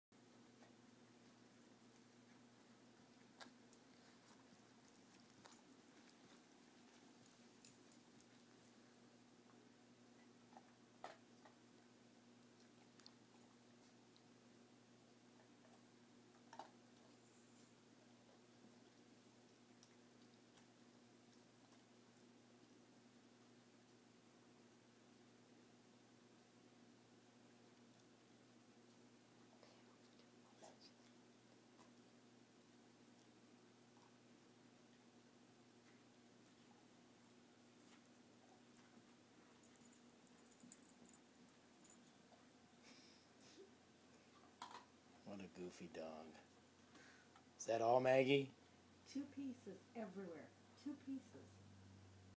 Maggies eating habits
Maggie picking up dog food she laid across the living room!